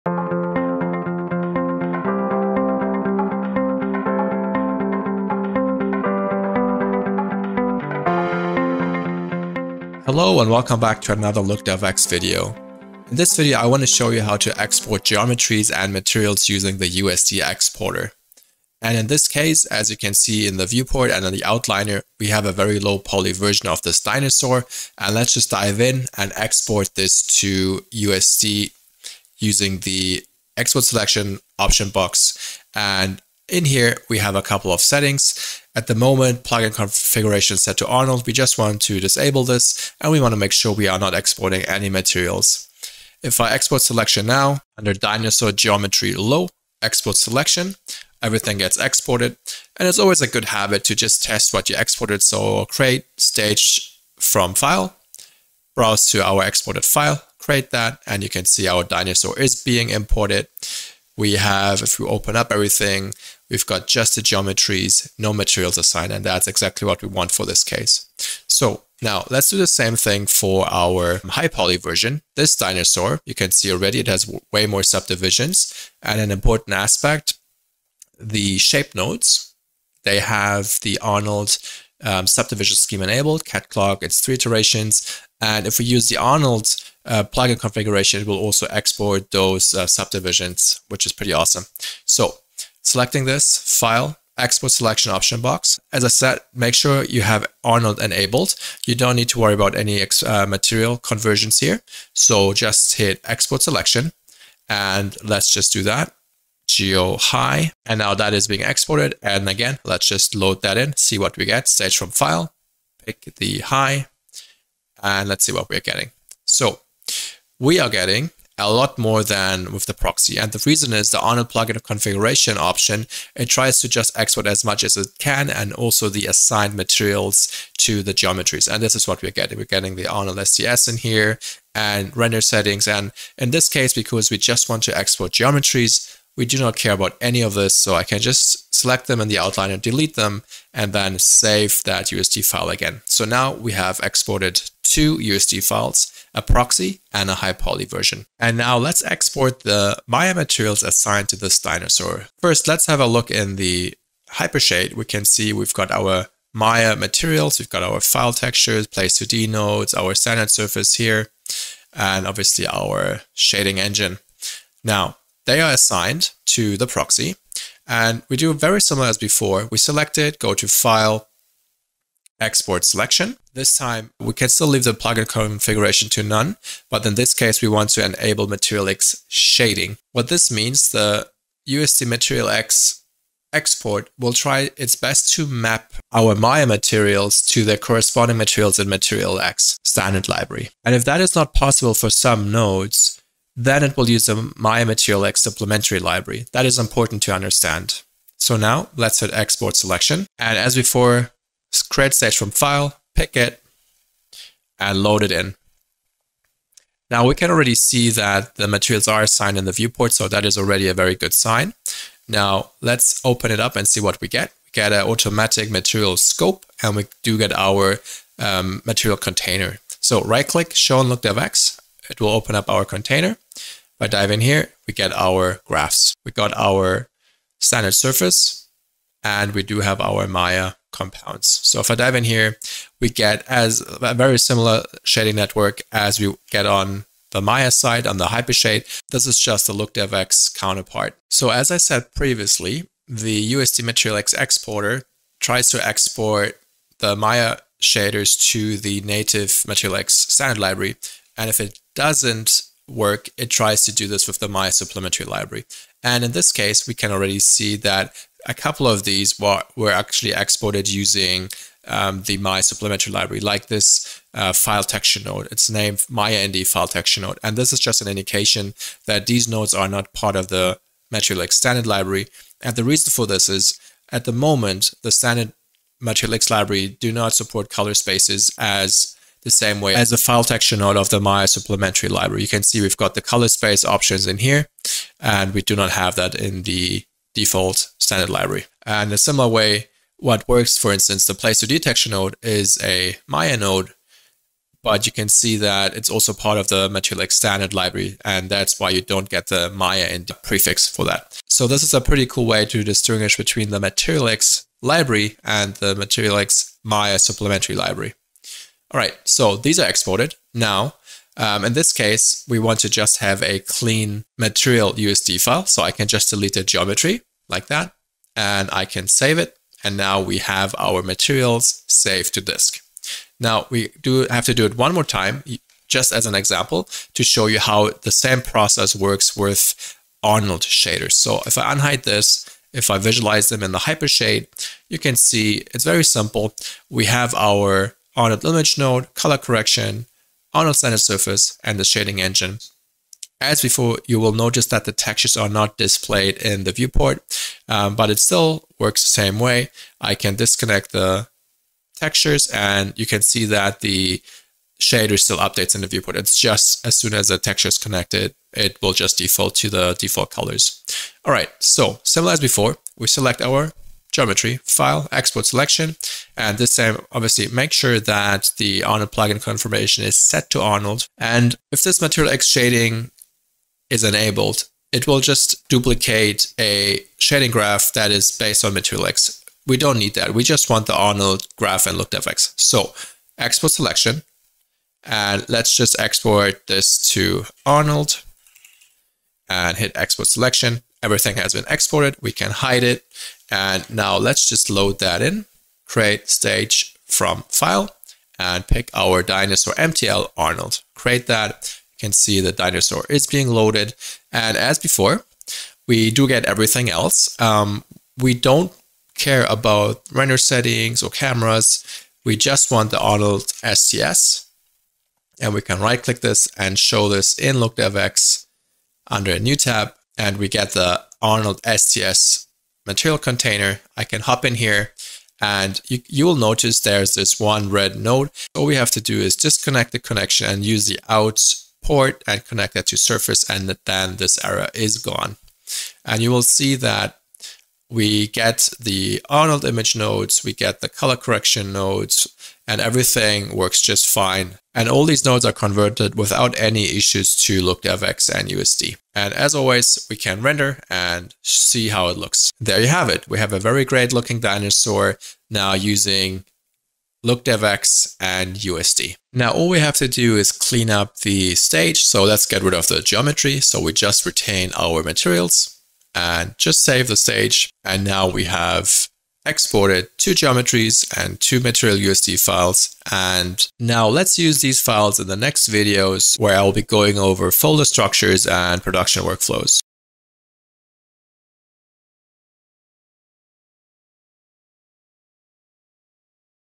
LookdevX in Maya Part 2 - Exporting Geometries & Materials
Seize the power of LookdevX, Autodesk’s new look development tool for creating OpenUSD shade graphs and authoring custom materials in Maya.

In this "How I Made It in Maya" video, Arvid Schneider (@arvidurs) exports geometries (proxy and high poly) and materials using USD Export.

LookdevX is Autodesk’s agnostic material authoring plugin that currently supports the OpenUSD (Universal Scene Description) data model natively in Maya.

If you have yet to download the assets from Part 1, find them here to follow along